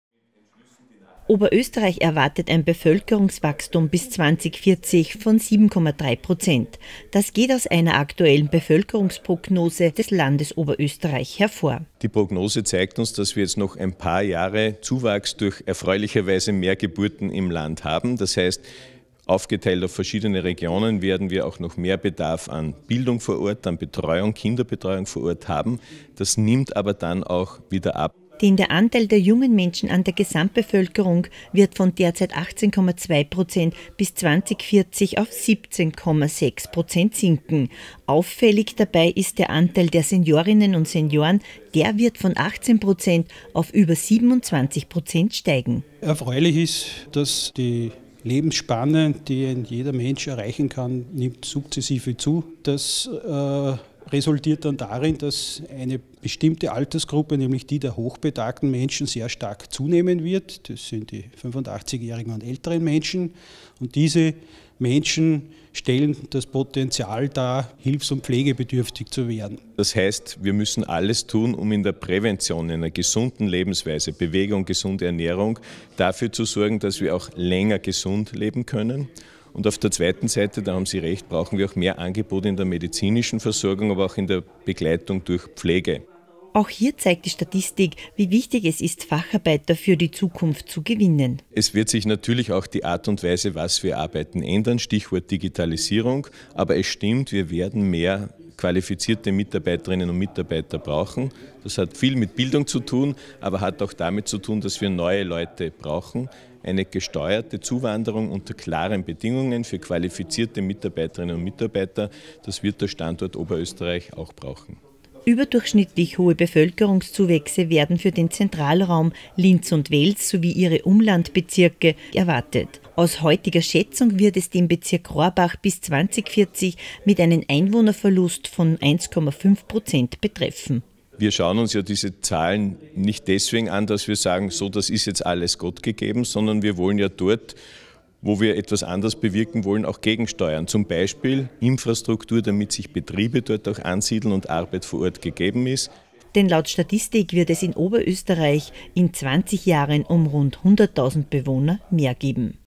Linz: Bevölkerung steigt bis 2040 auf 1,6 Millionen
Oberösterreichs Bevölkerung wird weiter wachsen! Laut aktueller Bevölkerungsprognose des Statistischen Dienstes des Landes Oberösterreich wird sich die Zahl der Einwohner bis 2040 auf fast 1,6 Millionen erhöhen. Am auffälligsten dabei: Der Anteil der Seniorinnen und Senioren wird von 18 auf über 27 Prozent steigen.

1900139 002 FB Pressekonferenz September 2019